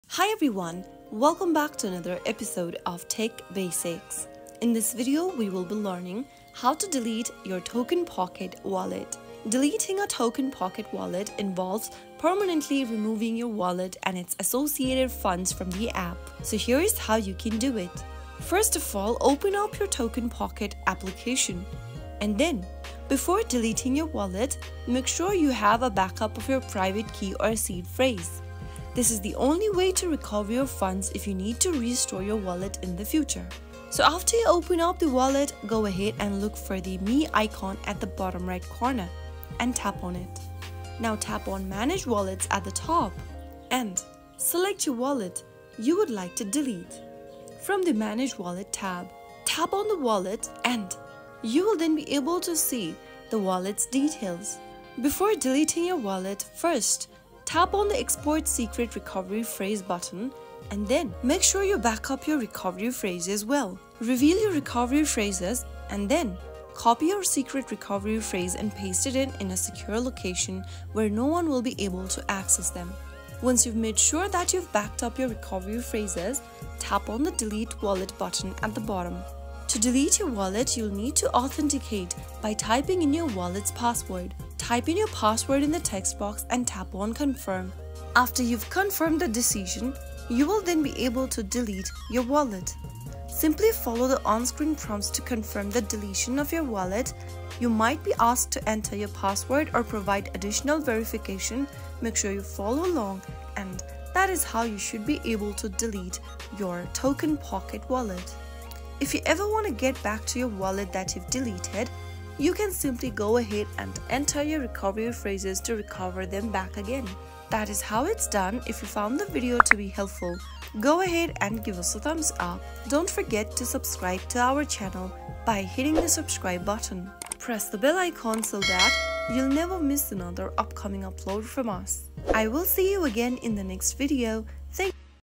How to Delete Token Pocket Wallet 2024?
🚫 Looking to delete your TokenPocket wallet? Whether you’re closing your account or switching to a different wallet, it’s important to understand the process for securely deleting your TokenPocket wallet. In this video, we’ll guide you through the steps to permanently delete your wallet and ensure your data is handled properly. 🔐